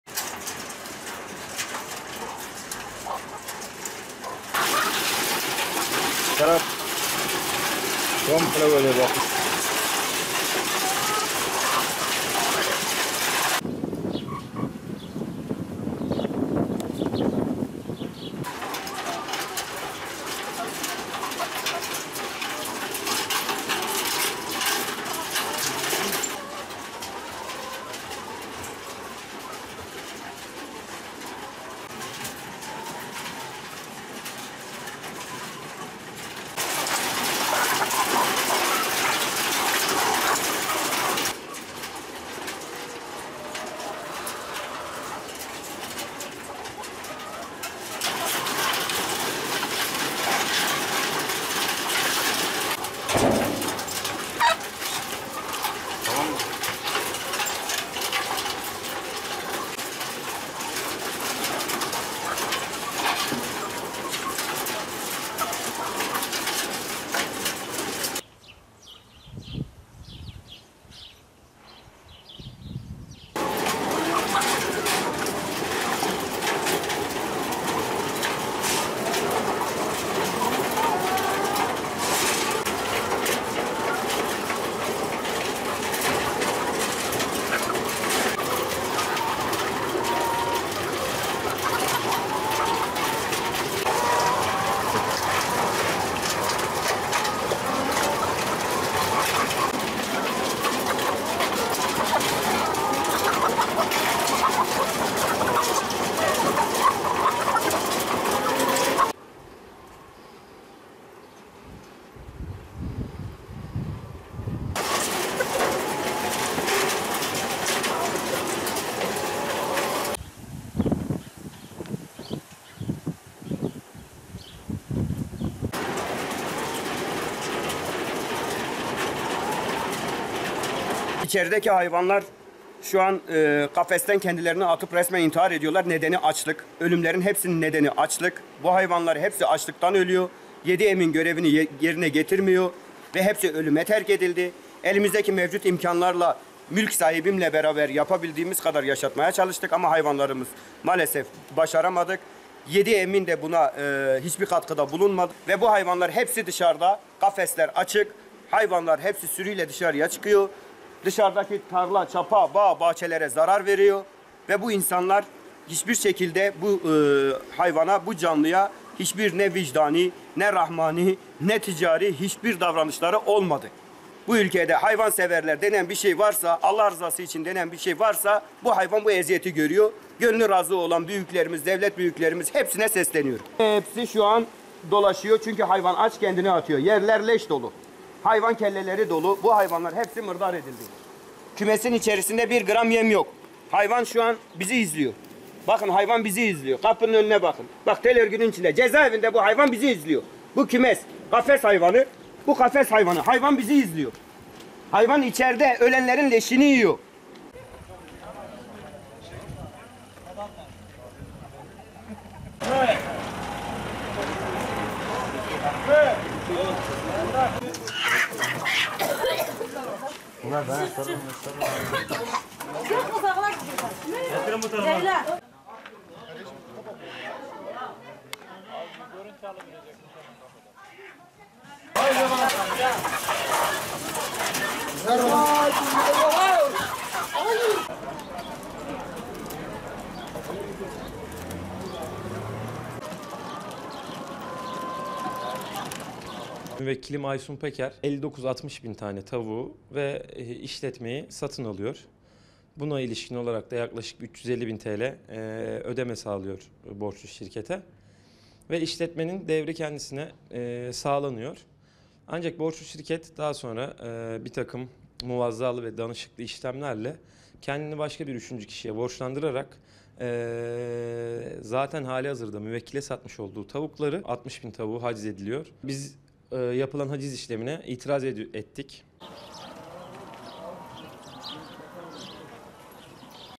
Denizli’de haciz katliamı: 40 bin tavuk telef oldu
Denizli’nin Acıpayam ilçesinde haciz koyulup yediemine teslim edilen 69 bin tavuktan, 40 bini açlıktan telef oldu, kalan yaklaşık 29 bin tavuk ise ölmek üzereyken kurtarıldı. Yedieminin ilgilenmediği ve ölüme terk ettiği iddia edilen binlerce tavuk, kafeslerin içinde günlerce aç susuz kaldı, bazıları açlıktan birbirini yedi. Duruma el koyan yetkililer, kalan 29 bin tavuğu kesilmek üzere Afyon’a gönderdi.